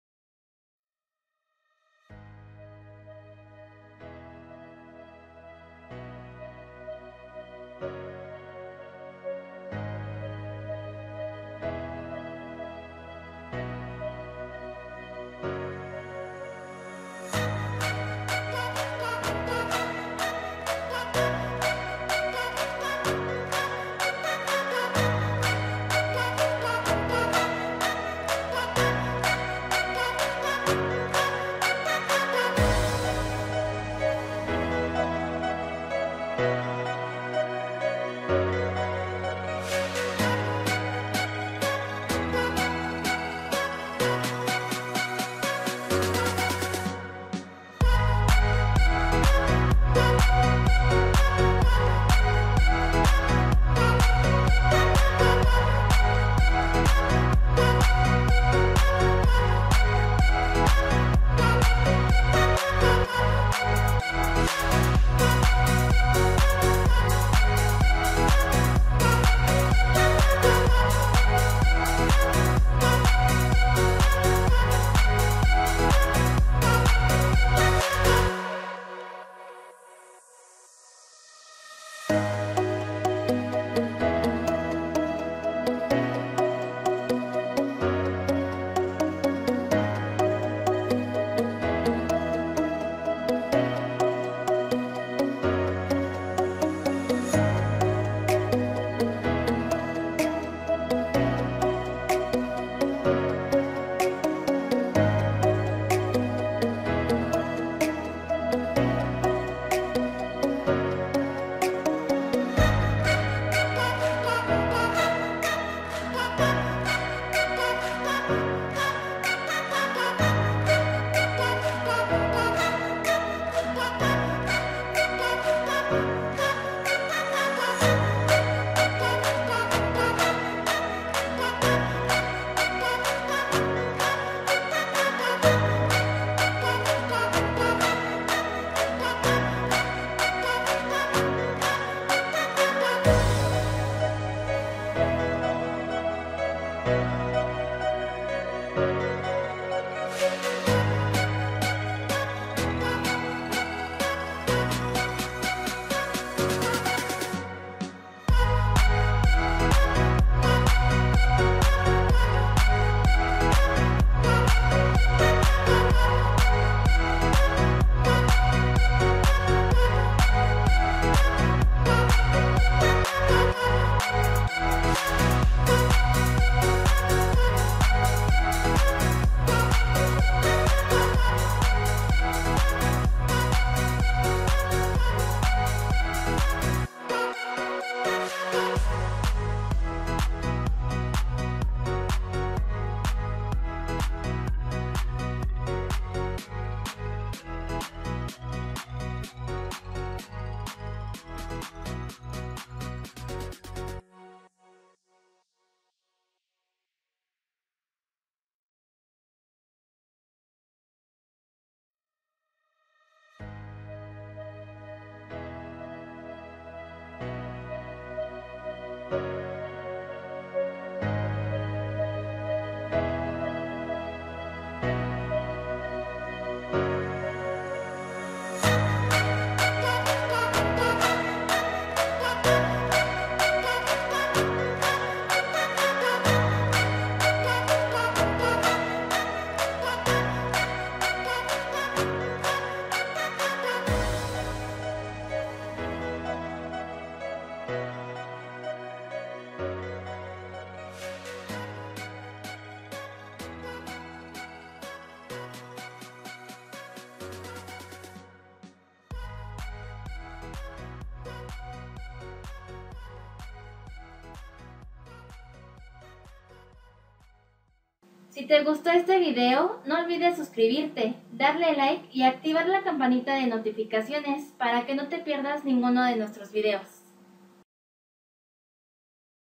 🌳 como dibujar un arbol paso a paso
En este video aprenderás a dibujar un ARBOL sencillo, fácil y rápido.